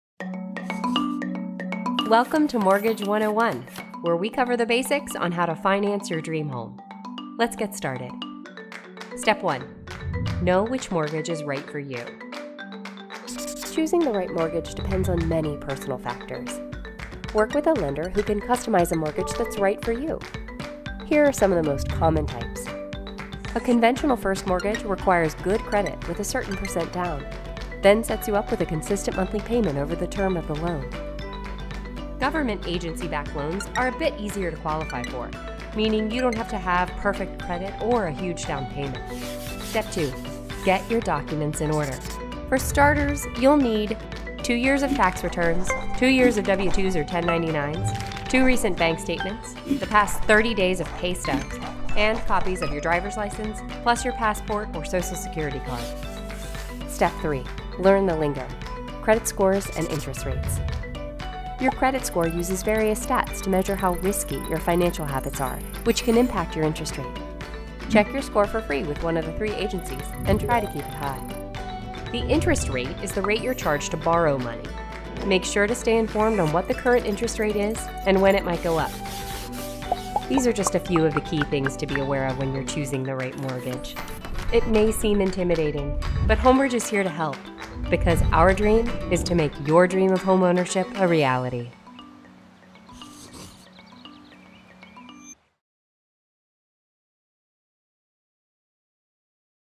Mortgage 101: The Basics of Mortgage Financing
Don’t know where to start when it comes to financing your dream home? Don’t worry, we’ve got you covered. Check out our video on Mortgage 101, so you can get up to speed with the basics of mortgage financing and the answer to the question: "what is a mortgage?"